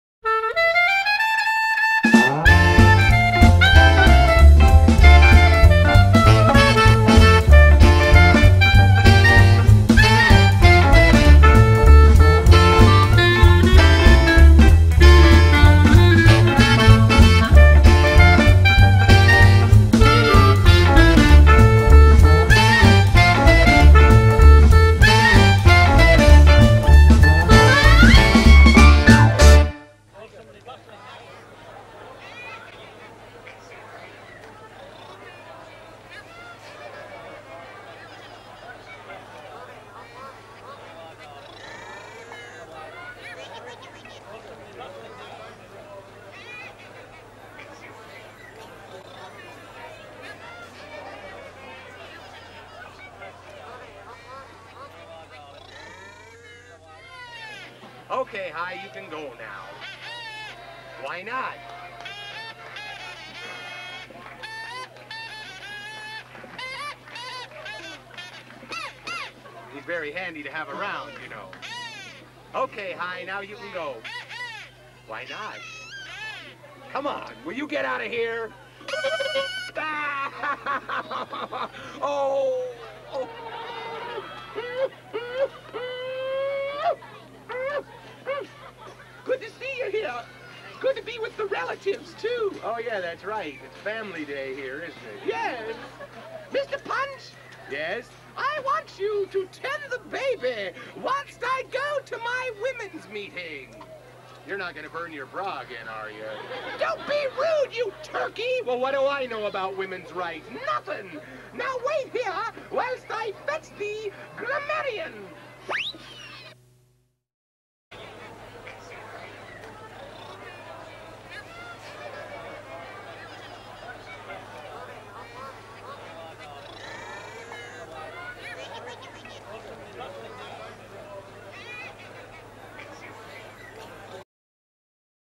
Steve Hansen The Puppet Man
Brief glimpses of Steve in action in 1974 at the Bankside Globe Punch & Judy Festival fundraiser in London.